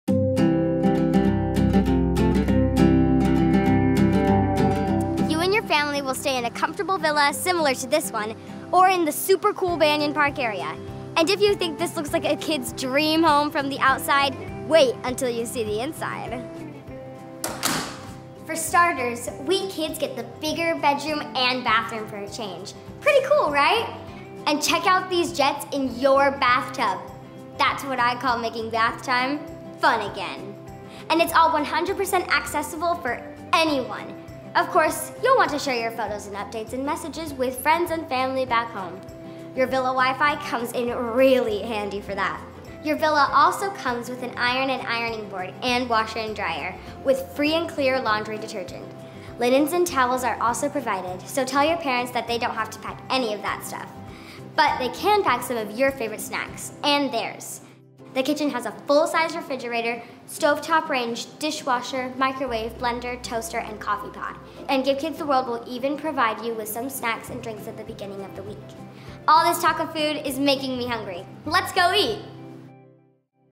3 – Accommodations
This segment introduces you to accommodations and amenities within the villas at Give Kids The World. Get to know your home away from home for the stay of a lifetime.